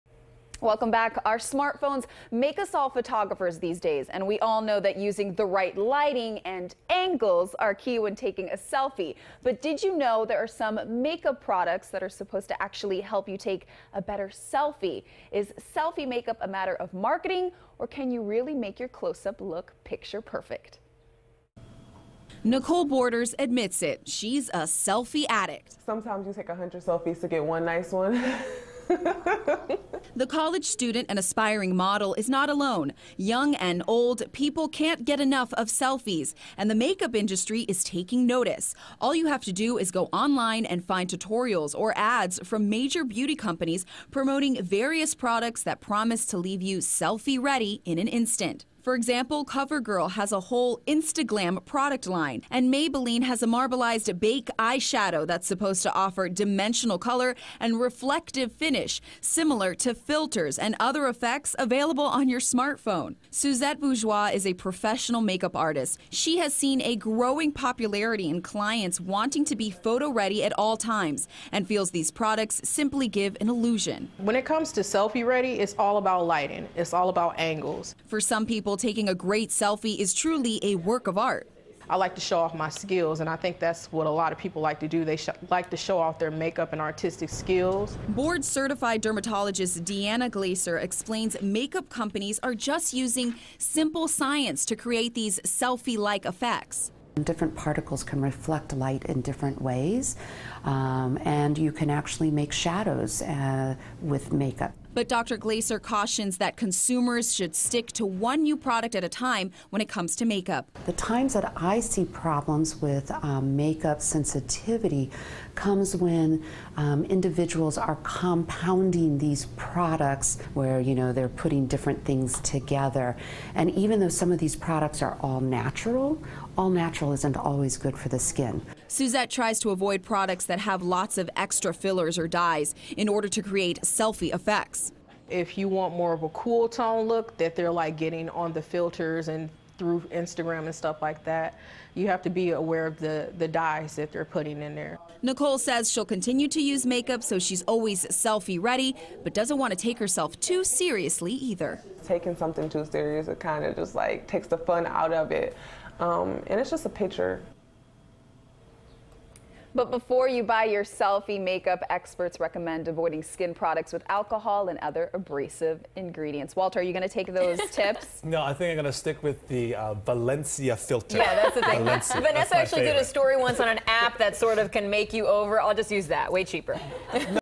Companies Now Marketing 'Selfie Make Up'
Supposed to make user look better in a variety of light conditions